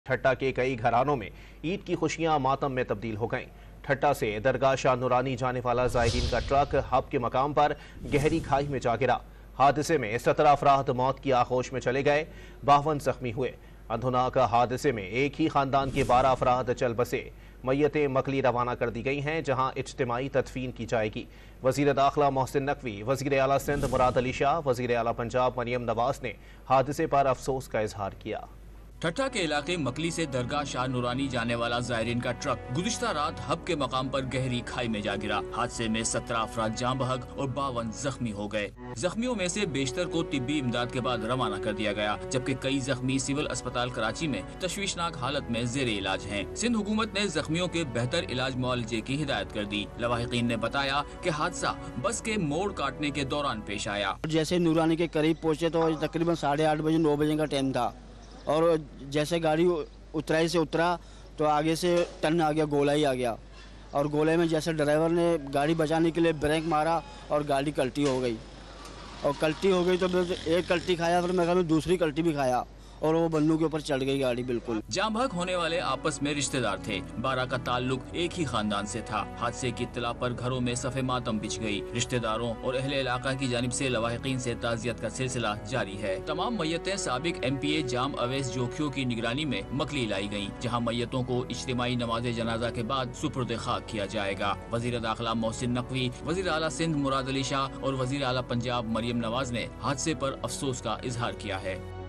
Truck Falls Into Ditch: Horrific Accident Of Pilgrims Traveling For Dargah Shah Noorani | Dawn News
عید کی خوشیاں ماتم میں تبدیل ہو گئیں
درگاہ شاہ نورانی جاننے والا ٹرک حادثے کا شکا ر